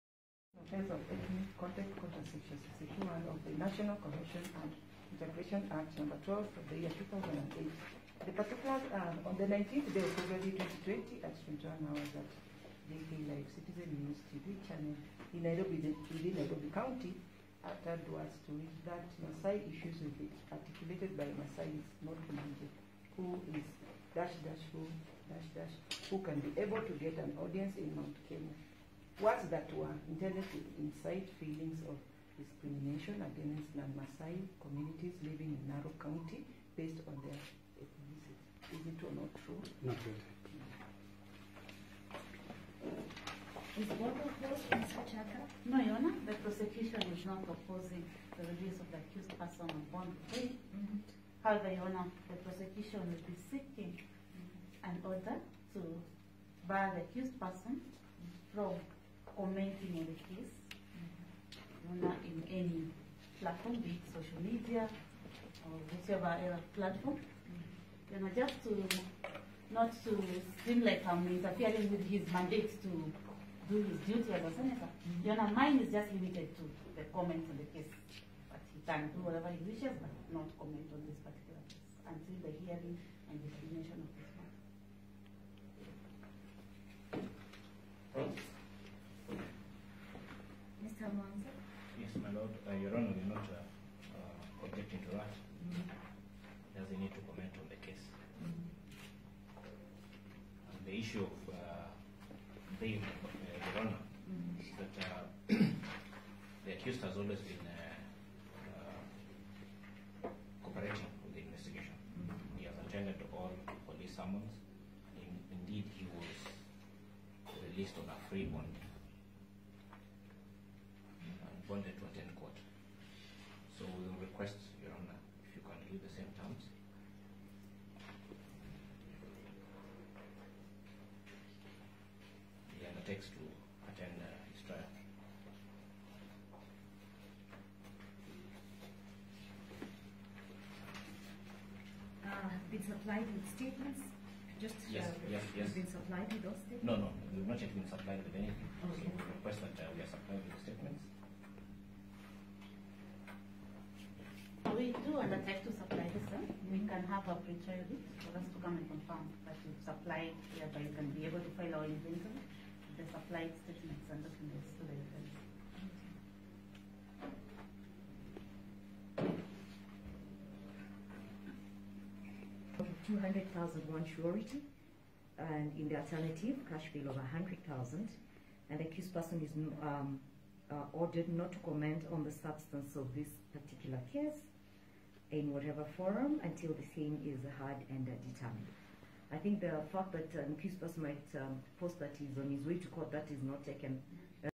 Narok Senator Ledama Ole Kina released on ksh 100,000 bail after being charged with ethnic contempt
The Senator had appeared before Milimani Chief Magistrate Martha Mutuku who dismissed the Senator’s application challenging the charge sheet.
As per the judge, objection lacked merit.
“I find that the court is called upon to consider it a charge is known in law….court has jurisdiction to hear the application …the charges are proper before the court,” she said.
He was released by court on a cash bail of Ksh. 100,000 and ordered him not to comment on the case until the matter is heard and determined.

Case is to be mentioned on July 30 for further direction